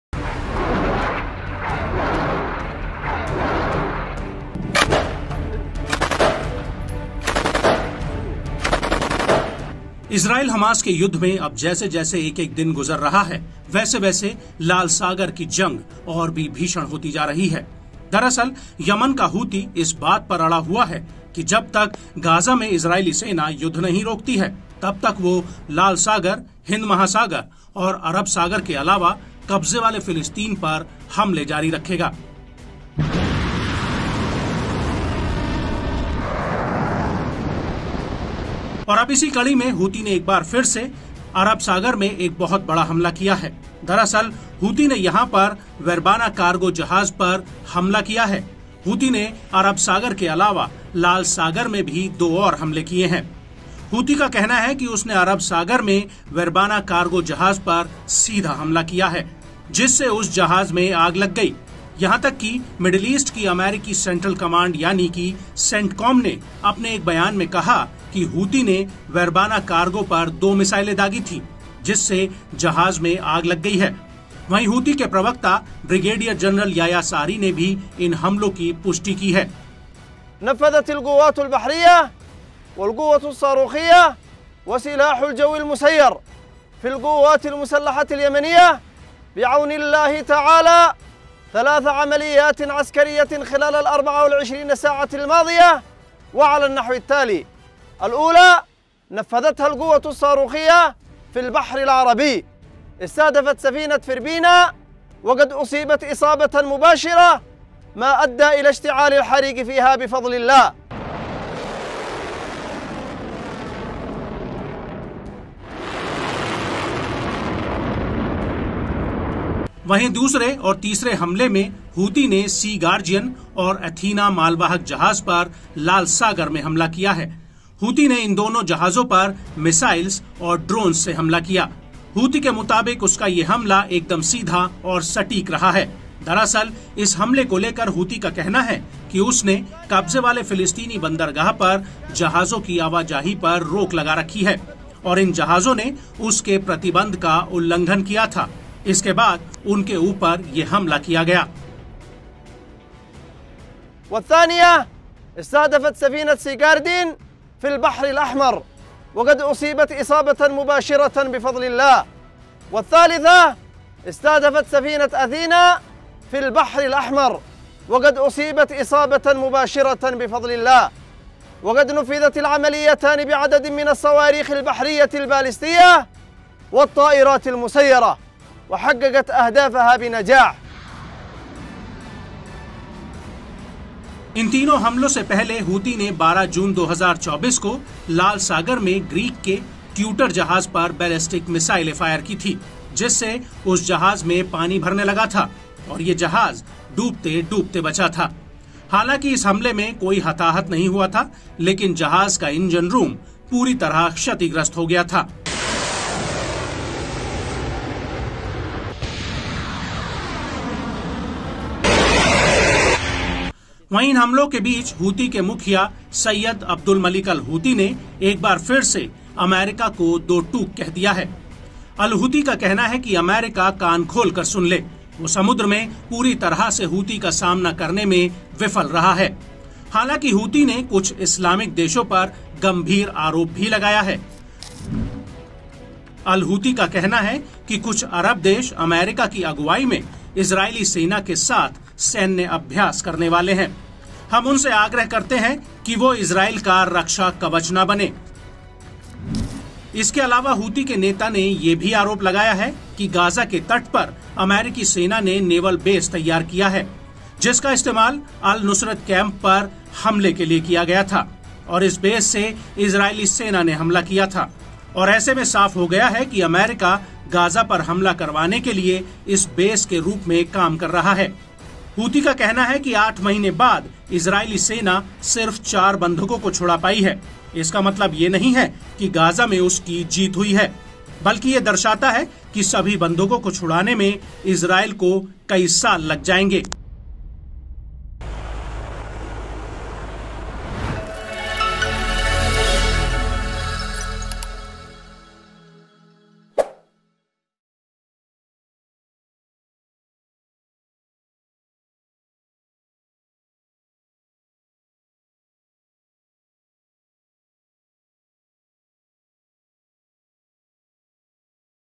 Israel Hamas War : अमेरिकी जहाज़ों पर 'खुफिया' अटैक | Gaza | Rafah | Palestine News | Top News
Arabian Sea Attack किया है. Houthi ने Red Sea Attack में भी दो जहाजों को निशाना बनाया है. Houthi Spokesperson Yahya Saree ने भी इस हमले की पुष्टि की है. उन्होंने अपने बयान में कहा कि हूती की नौसैनिक सेना, मिसाइल फोर्स और ड्रोन फोर्स ने अल्लाह की मदद से, पिछले 24 घंटों में 3  हमले किए हैं.